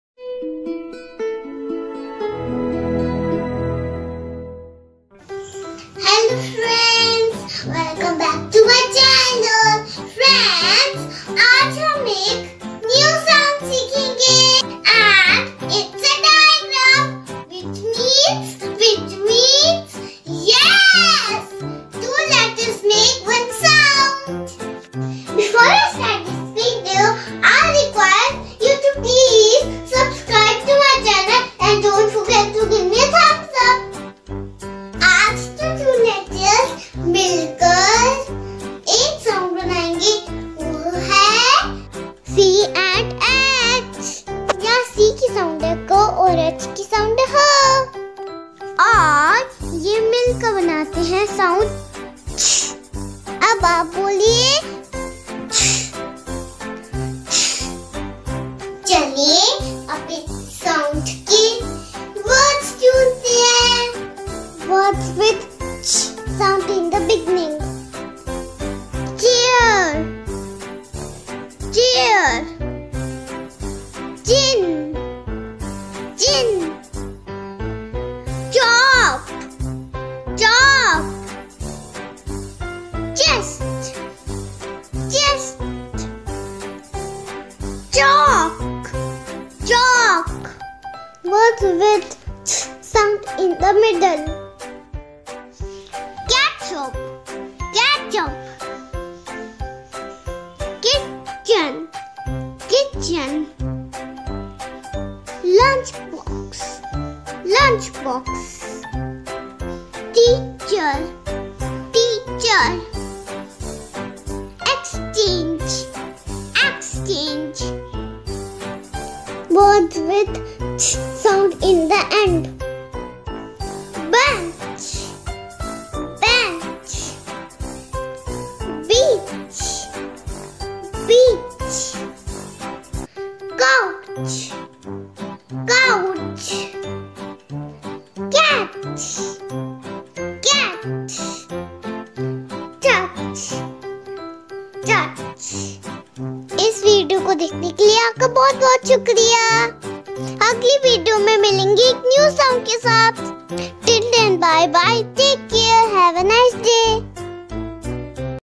Digraphs CH 'च' |Consonant digraph "ch" समझे हिंदी में | Easy to Learn | English For beginners
Nayra in this video is helping us completely understand the concept of Digraph "CH"_ A digraph is when two letter sounds or phonemes combine to make one sound or phoneme_ When the "C" sound combines with the "H" sound they create a unique sound of "CH"_ So learn about the Digraph "CH" as you speak along and follow Nayra as she helps you learn all about Consonant Digraph "CH"

# Digraph
# Consonant
# CH
# Digraph ch